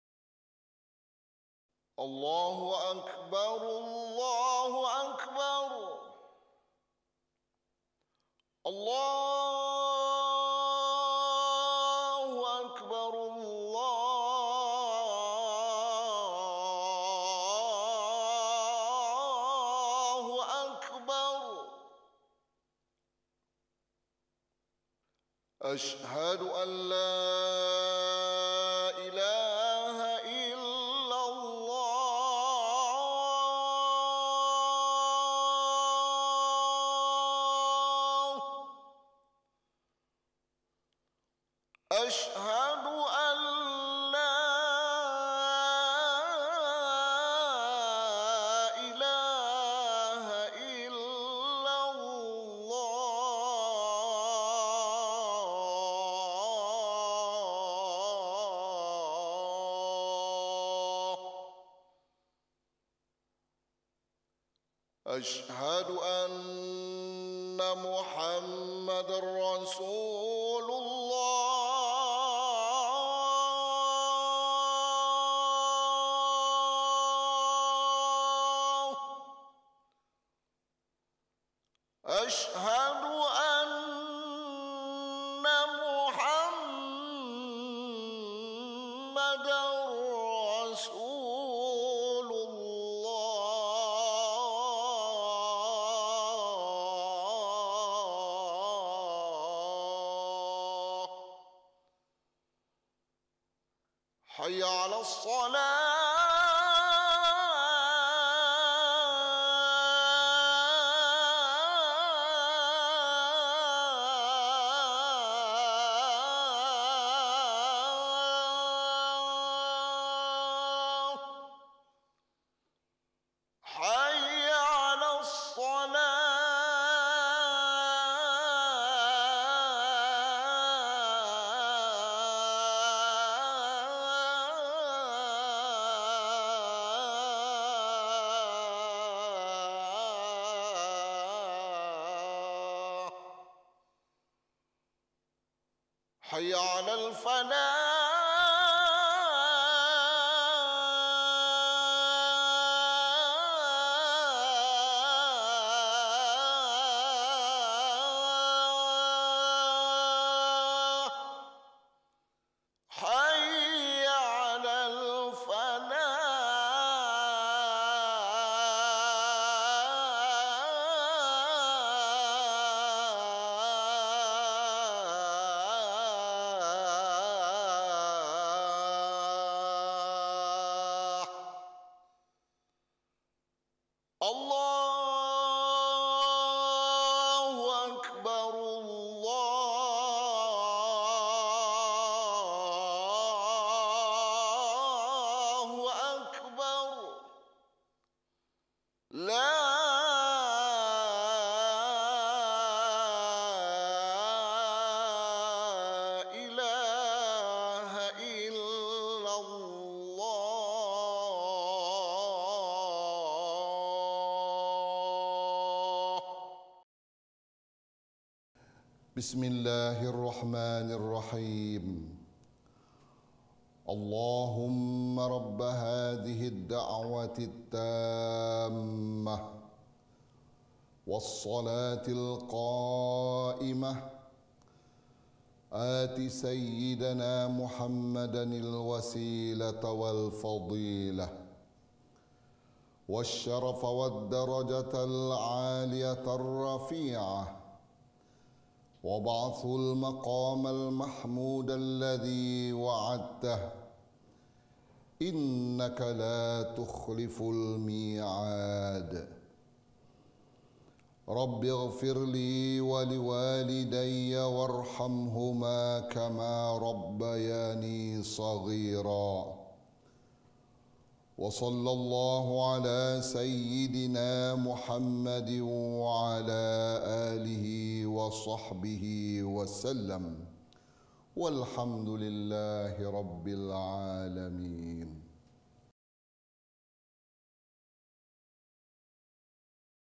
Azan Maghrib Negara Brunei Darussalam
Muazzin : Begawan Mudim Hj Metassim bin Hj Metussin
Terbitan Bahagian Media (Penyiaran Da'wah), Pusat Da'wah Islamiah dan Radio Televisyen Brunei.